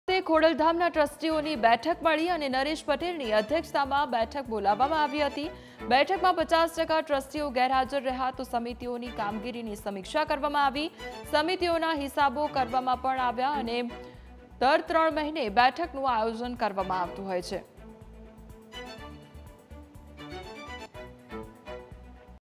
રાજકોટ: કાગવડ ખાતે ખોડલધામમાં નરેશ પટેલની અધ્યક્ષતામાં બોલાવાઈ બેઠક ,
બેઠકમાં 50 ટકા ટ્રસ્ટીઓ રહ્યા ગેરહાજર